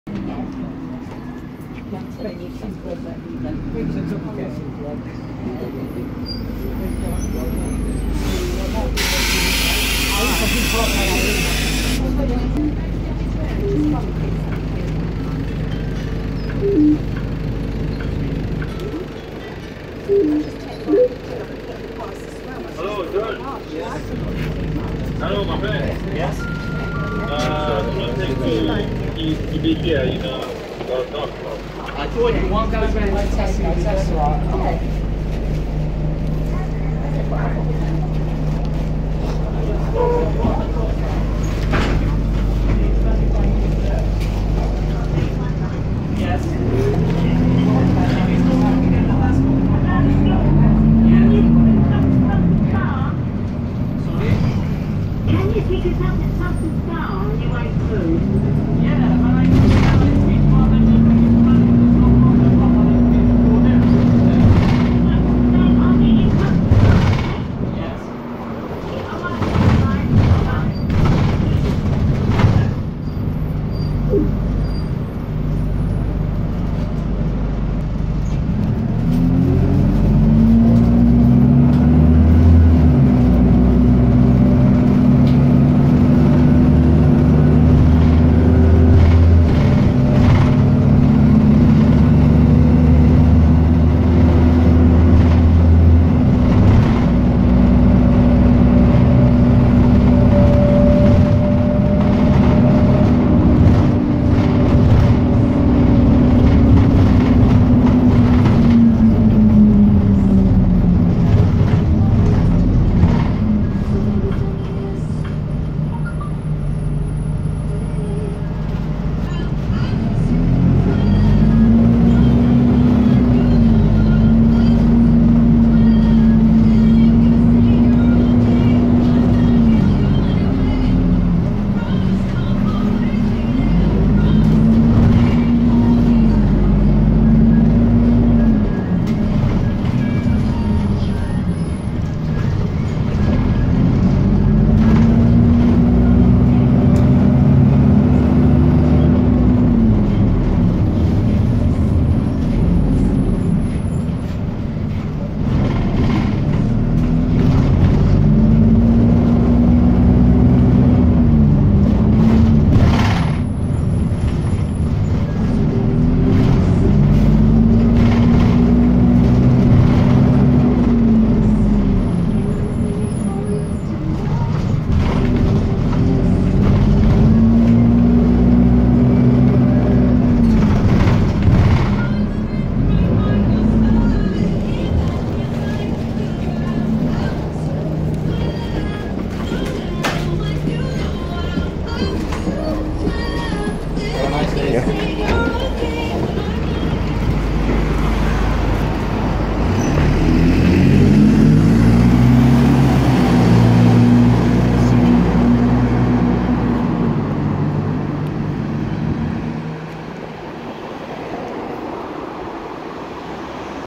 Trustybus Dennis Dart/Plaxton Pointer HW54 BTZ
This bus is on the 242 since the 242 uses them on Sundays and Bank holidays and also it has an iBus which is cool. The font colour was green so there wasn't a chance to see it it's so sunny and hot

Thanks